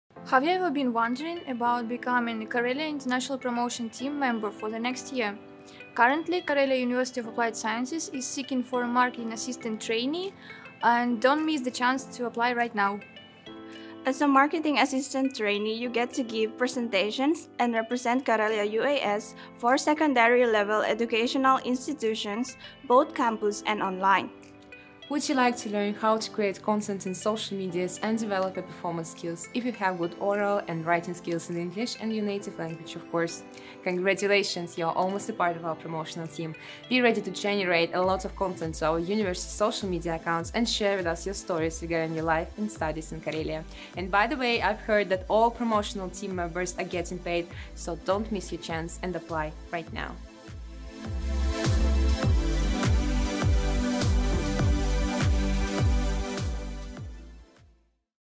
Join Karelia UAS International Student Promotion Team for 2020 - 2021!
On this video Karelia UAS international students share their ideas about why you, Karelia UAS international student, should join International Student Promotion activity for Academic Year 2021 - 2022.